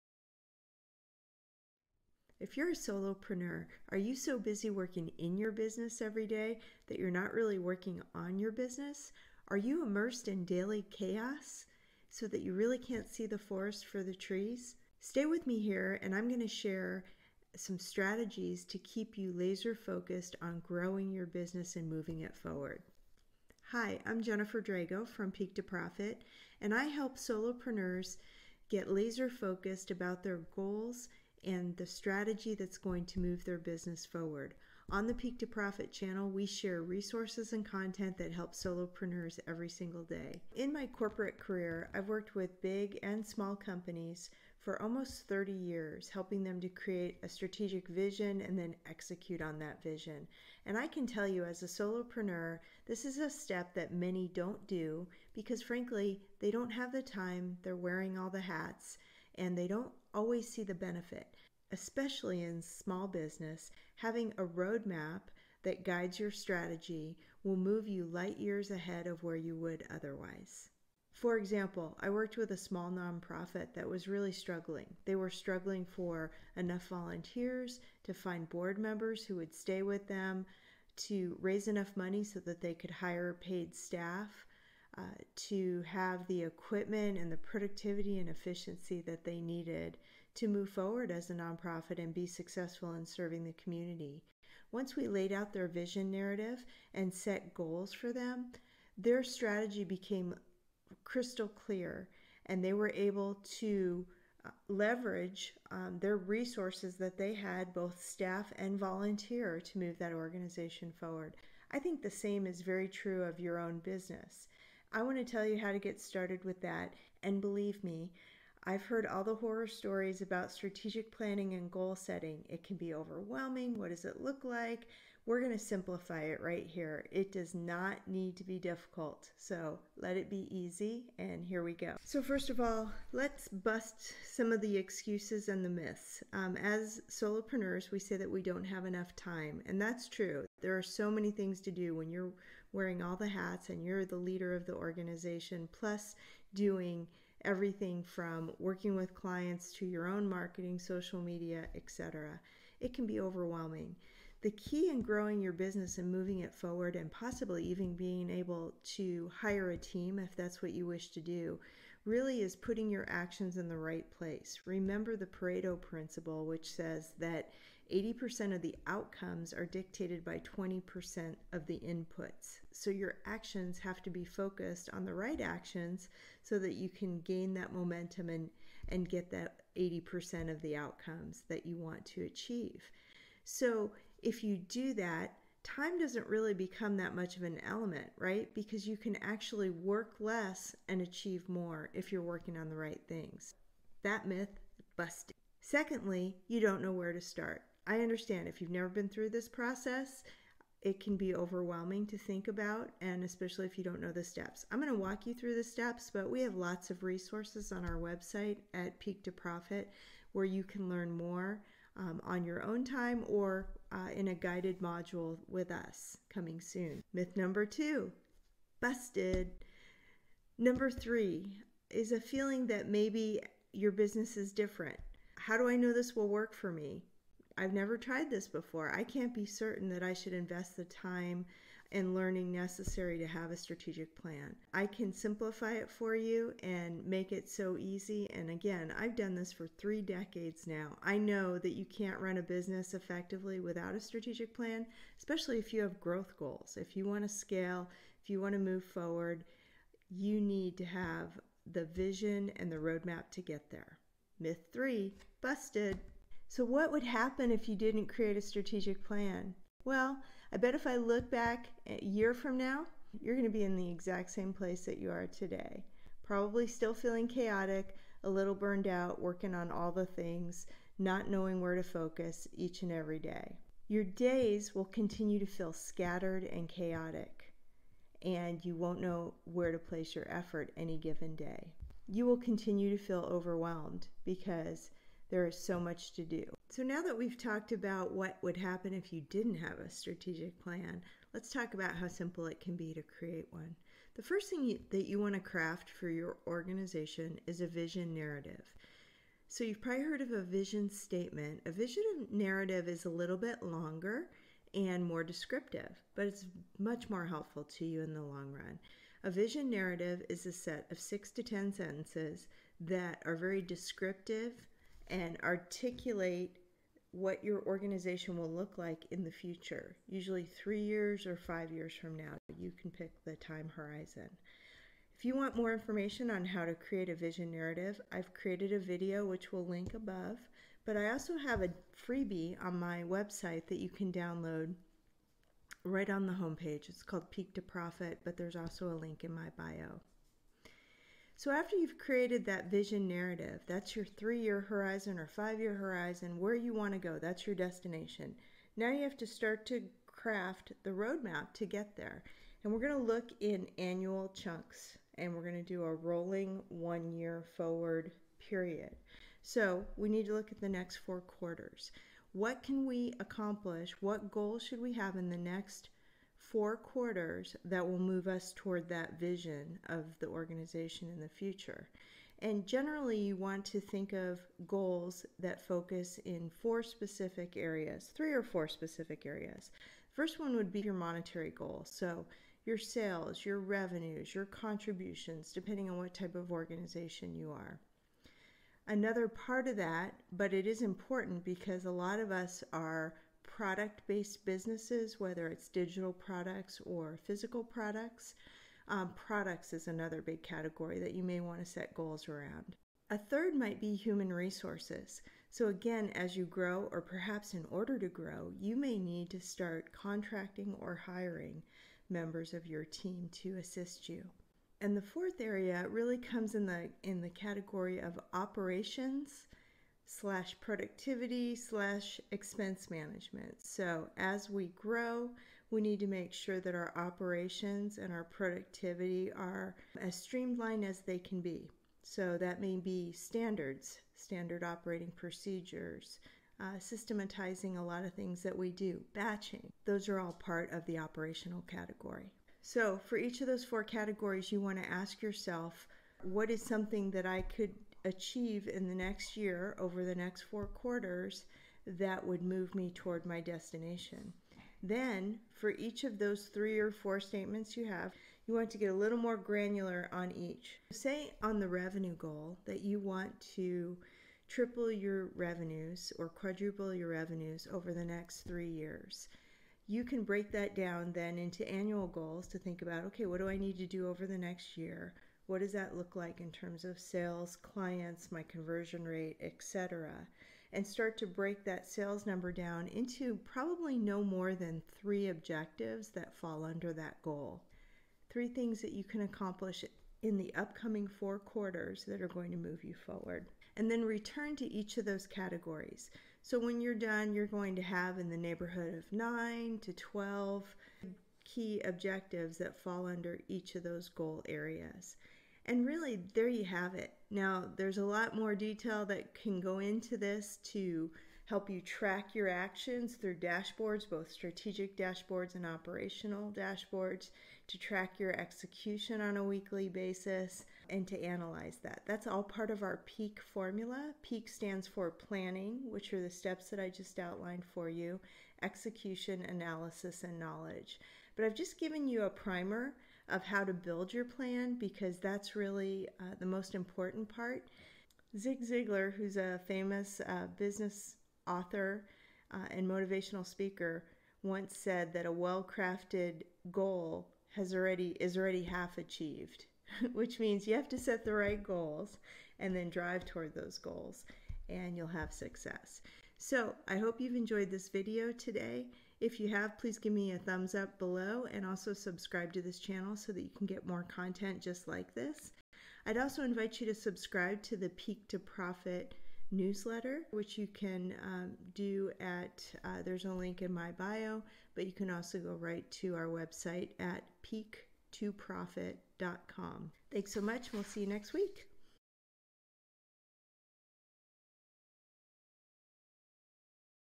How to Create a Simple Strategic Plan
Most entrepreneurs and small businesses do not develop strategic plans to guide their business strategy. As a result, they exist in a chaotic environment when their efforts could be better focused and have better results.

Why? They believe they don't have enough time, they think the process is too complex and they think that perhaps their business does not need a strategic plan. In this video, we debunk these myths but also provide a simple process to create a vision and strategic plan.

🤓 Want to know the 5 myths of successful entrepreneurs? Enjoy this free resource.